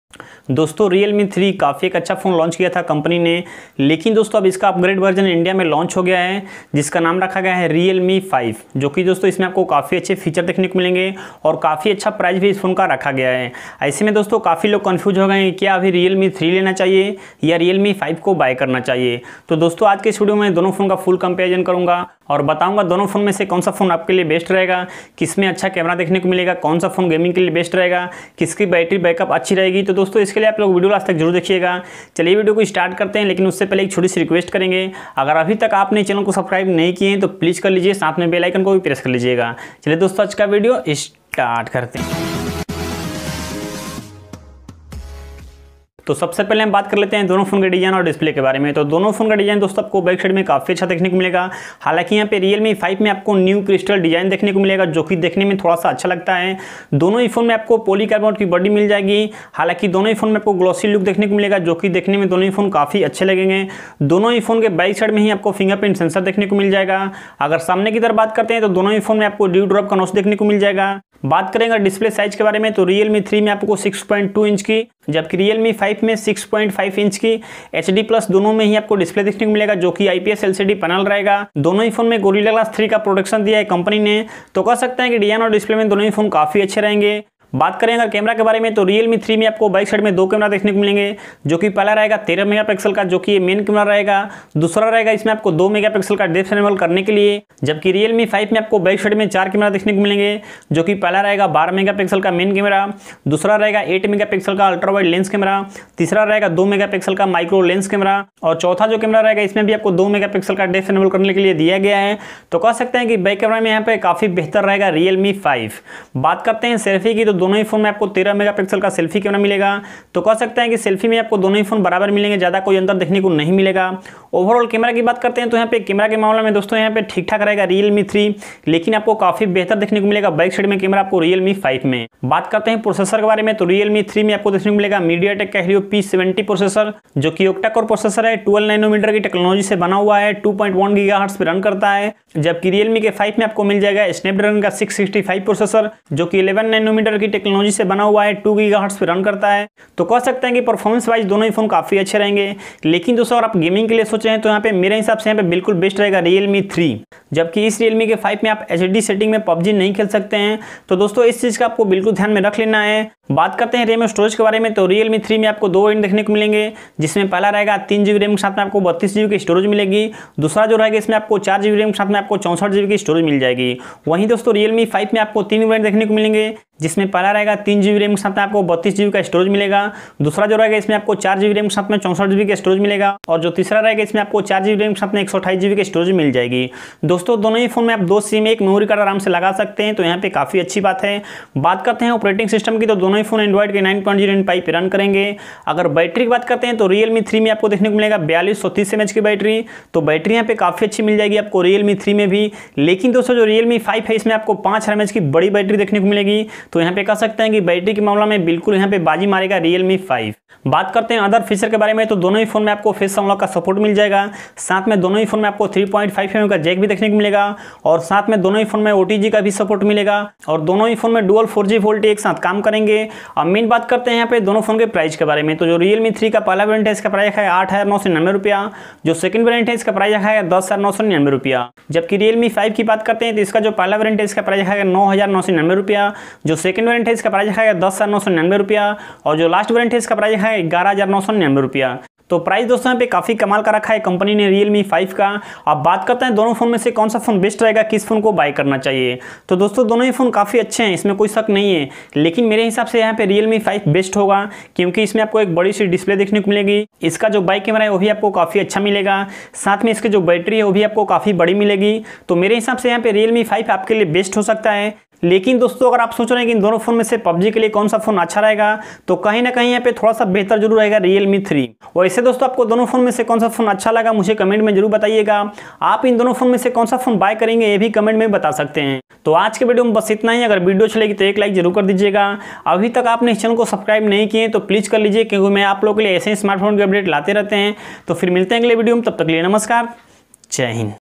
Realme 5 vs Realme 3 - Full Comparison,Camera,Performance,Battery,Display,Price ।। Who is Best?
Numskar dosto pyar banaye rakhna🙏

Dosto aaj ke video me Realme 3 vs Realme 5 specification Comparison kiya hu, ummid hai video pasand aayegi..

Topic in video :-

Realme 3 Full Specification
Realme 5  full Sepecification
Realme 3 vs Realme 5 full Specification Comparison in hindi

Any Product Buy From Amazon
With Best Offer and Help me